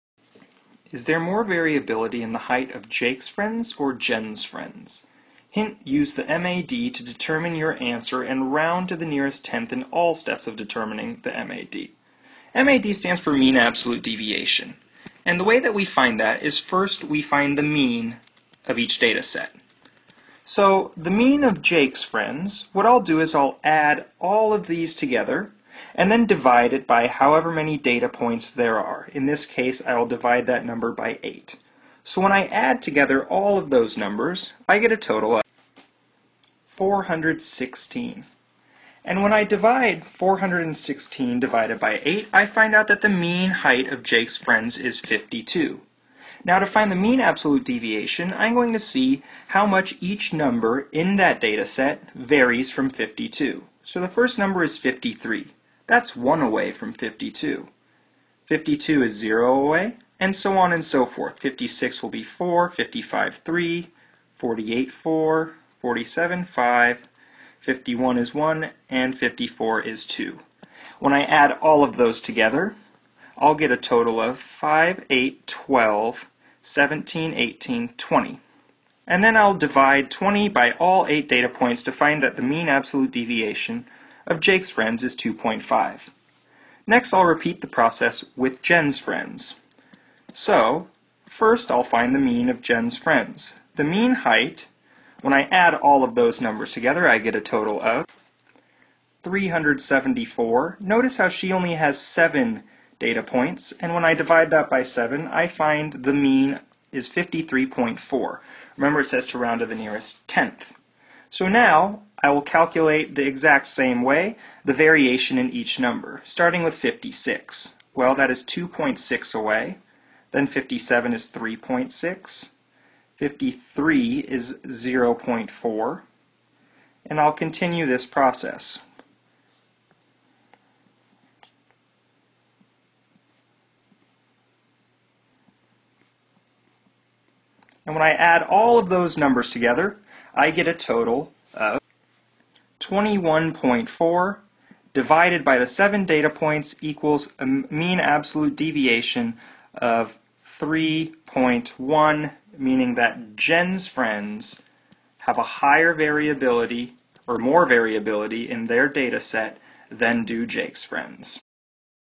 [7.SP.4-2.2] Comparing Data Sets - Common Core Standard
Use the mean absolute deviation/interquartile range to draw informal comparative inferences about two populations